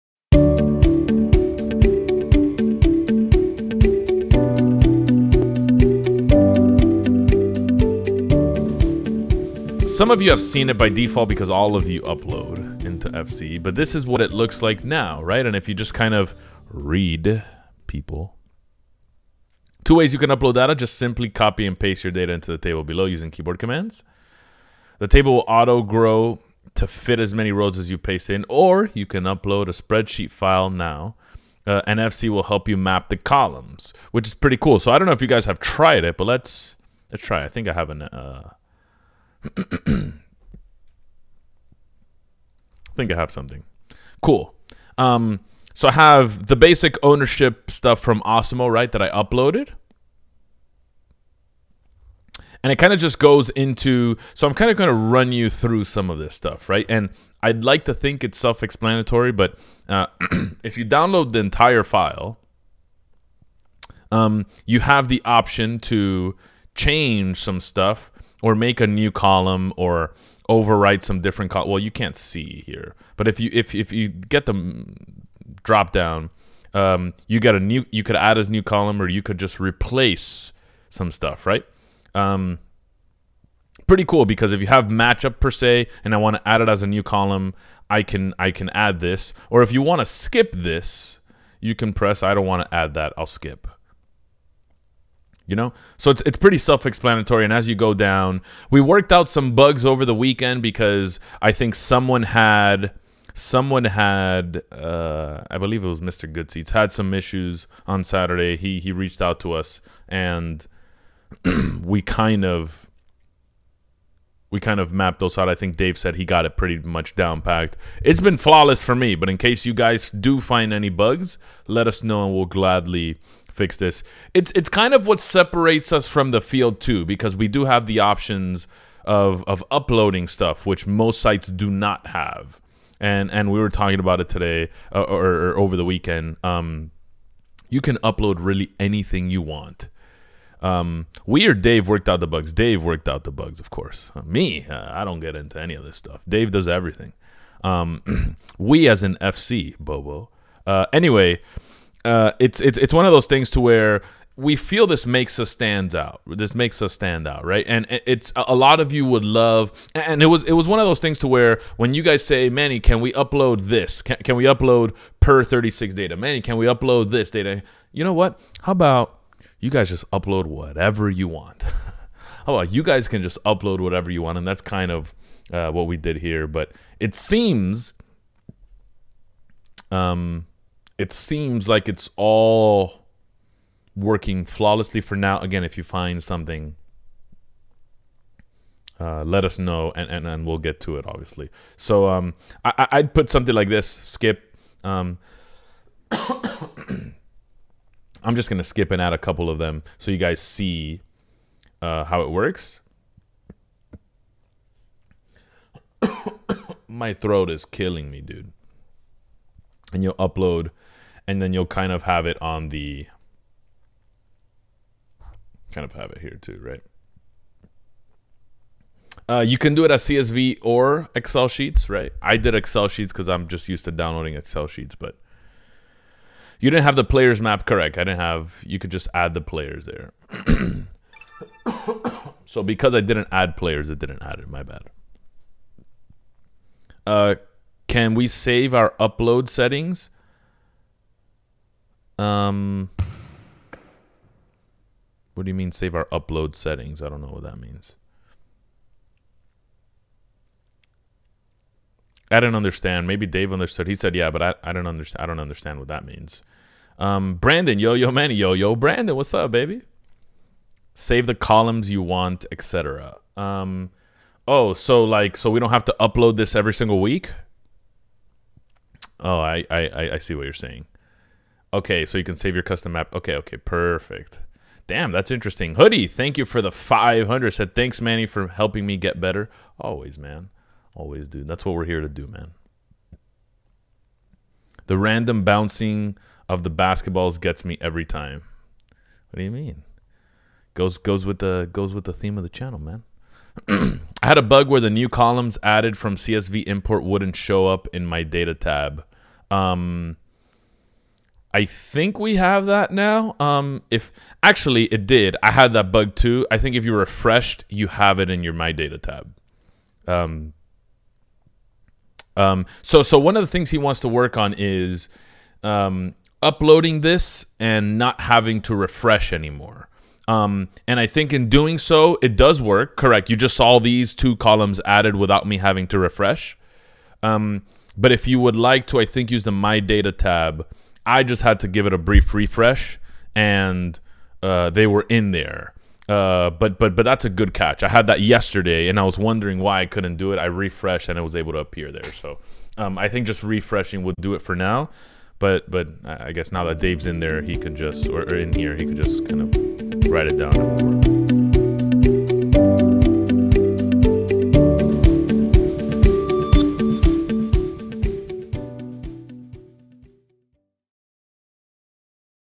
FantasyCruncher Tutorials: How to Upload Data & Upload by CSV
FantasyCruncher's own Manny Lora walks us through the "Upload Data" tab on FantasyCruncher.com and a new feature that lets you upload your data directly from your CSV.